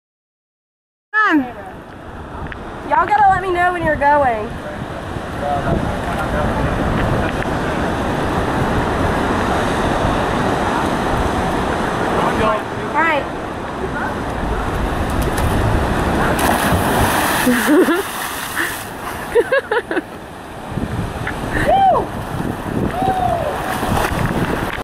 Jumping in the Mediterranean Sea.
Trip to Cinque Terre. In the Fourth town Owen and I jumped in the water.